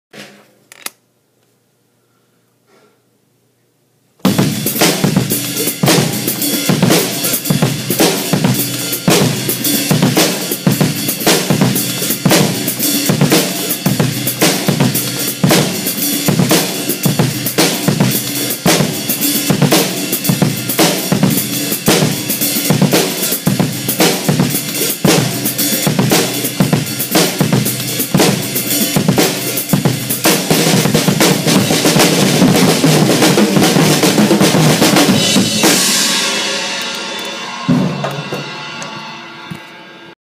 Double Paradiddle Foot Groove
like the title says its a double paradiddle.  first 2 notes are on the bass pedals then 2 notes on the hihats, then i play the diddle by splashing the hat with your heel and closing the hat with the toe of the same foot.  then it will repeat starting on the opposite foot.

because it repeats every 12 notes, if u play in 4/4 over top it makes a nice polyrhythm.

if anyone requests another video of me playing this slower ill be glad to do that.